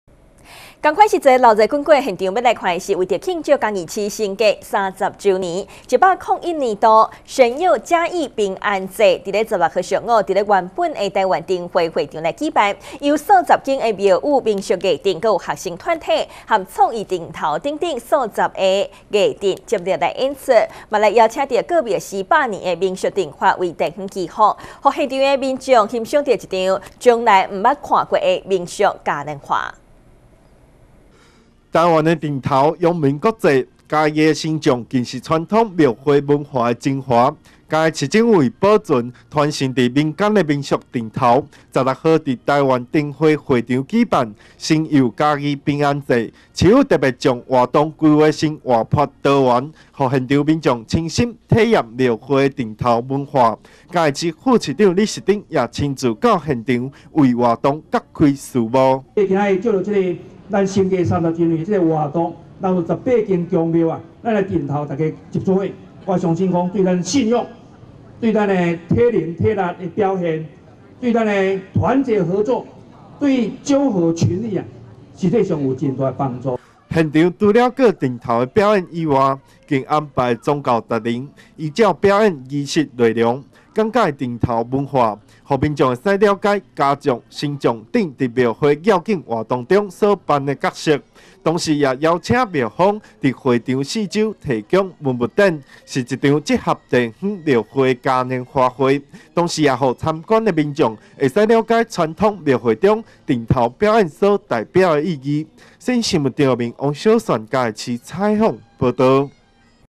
世新新聞 神祐嘉藝平安祭 嘉義市盛大登場
神祐嘉藝平安祭 嘉義市盛大登場

為慶祝嘉義市30升格30周年，101年神祐嘉義平安祭，16號上午在原台灣燈會會場舉辦，由數十間廟宇的民俗藝陣、學生團體和創意陣頭等等數十個藝陣接力演出，並邀請各廟寺百年的民俗陣法為地方祈福，讓現場觀眾欣賞一場從來未有的民俗嘉年華會。